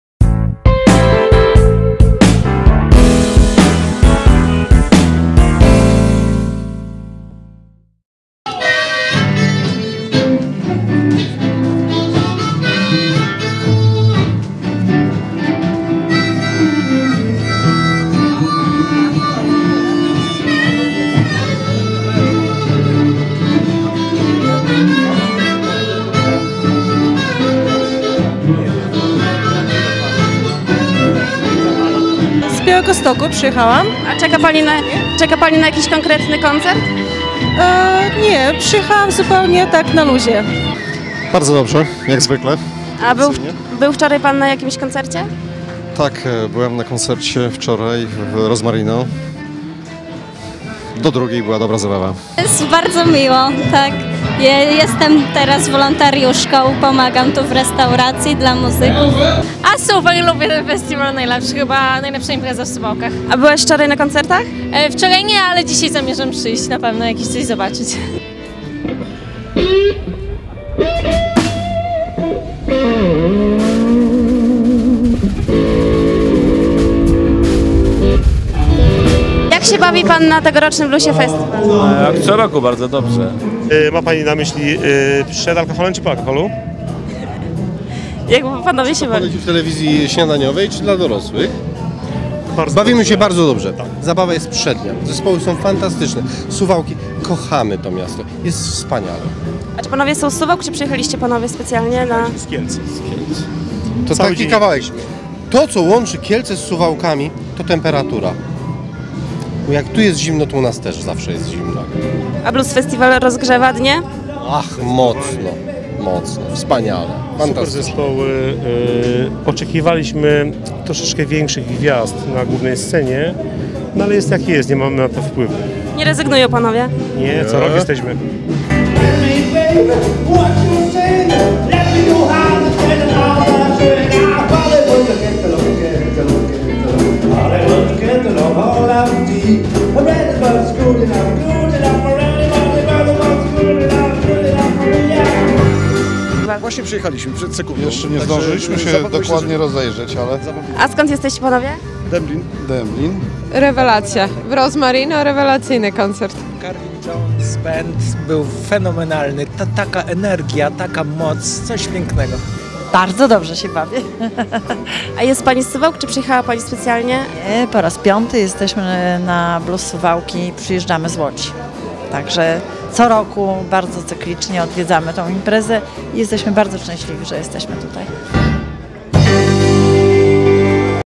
Blues na śniadanie
W ramach Suwałki Blues Festiwalu w kilku suwalskich lokalach odbywają się bluesowe śniadania